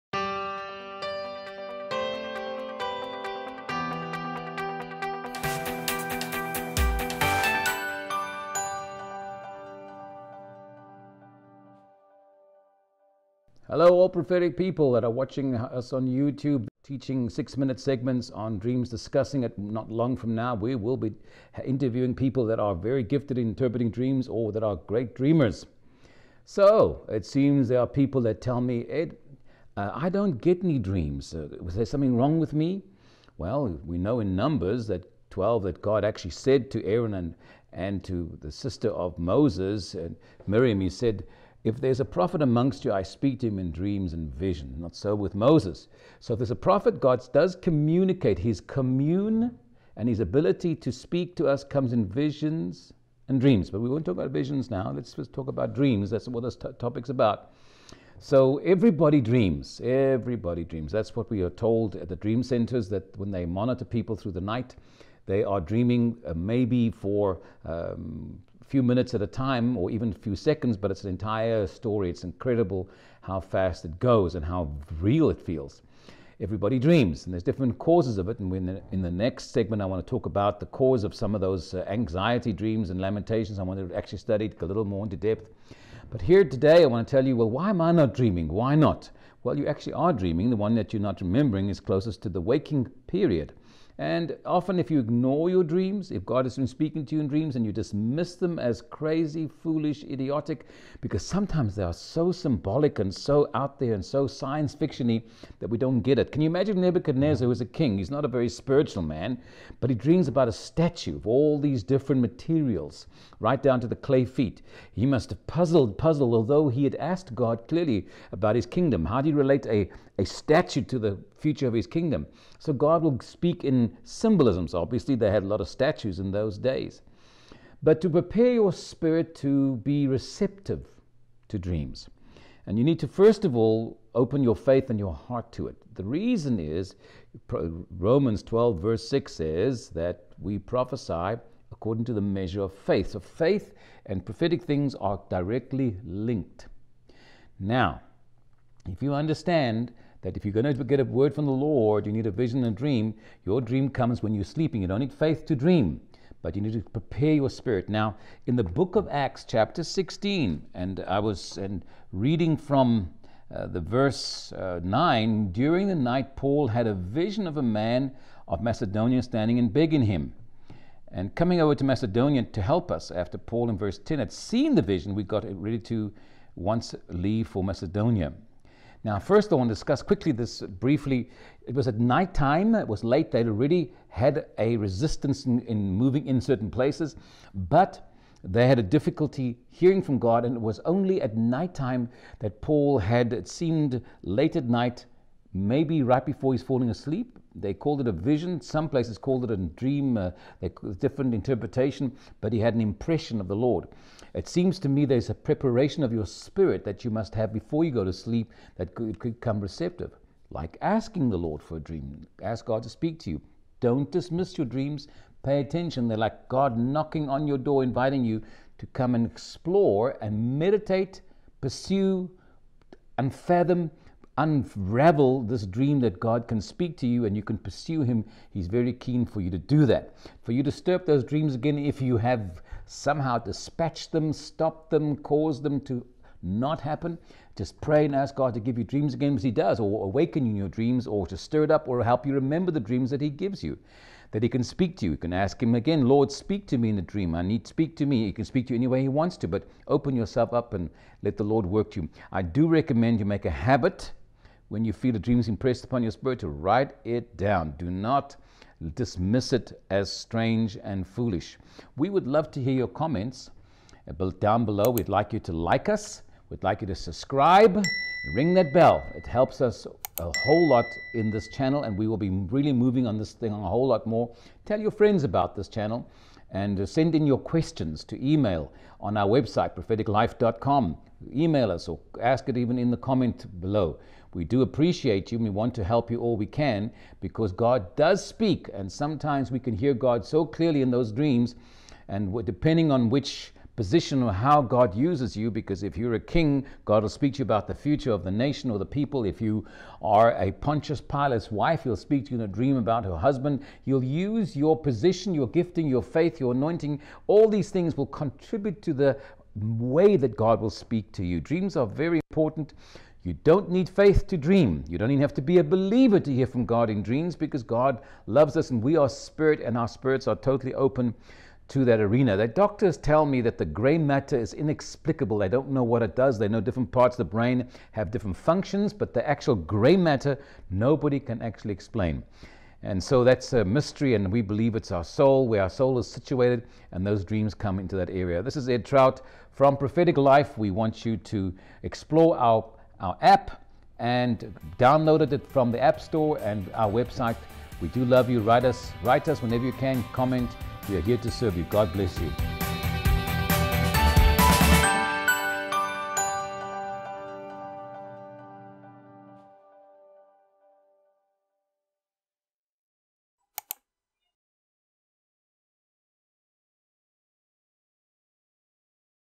Dream Again
Prophet Ed discussing dreams. Numbers 12, God speaks through dreams!
Faith and prophetic things are linked together.